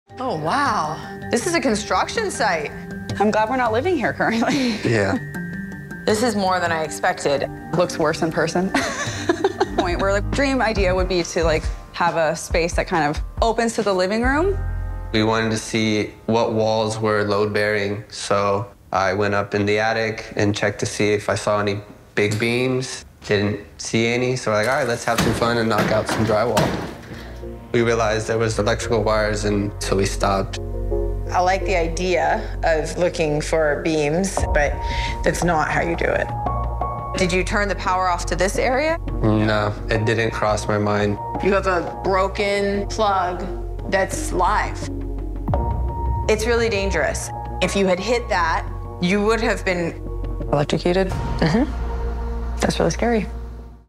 Couple Makes A HUGE MISTAKE When Tearing Down A Wall
First-time buyers Kendall and Broderick began renovations on their home. Things took a turn for the worse as they made the huge mistake of tearing down a wall that could've electrocuted them.